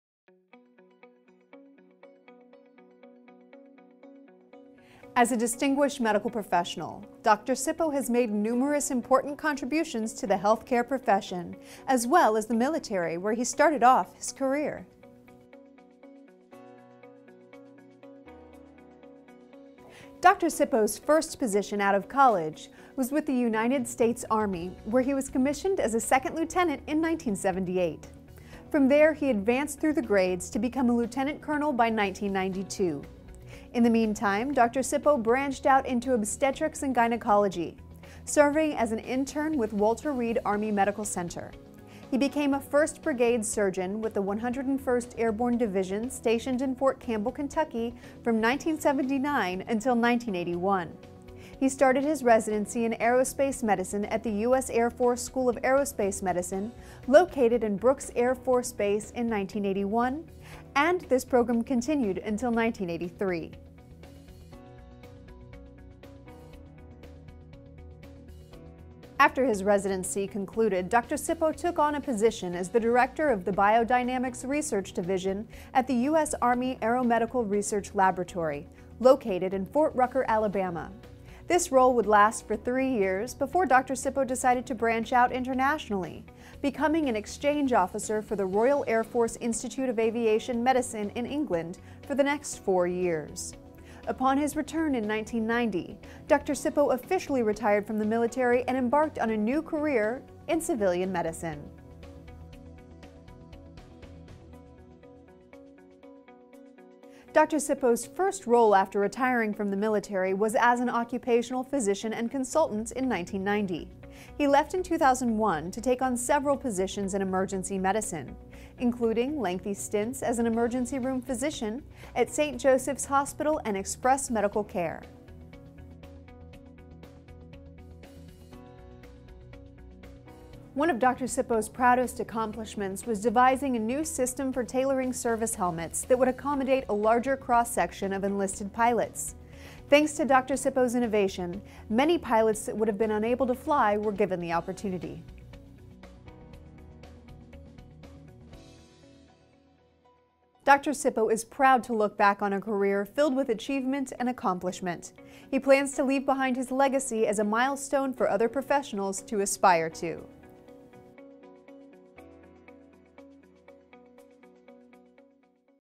Arthur Sippo Served the Military as a Medical Professional
Dr. Sippo proudly served his country through the military as a medical professional.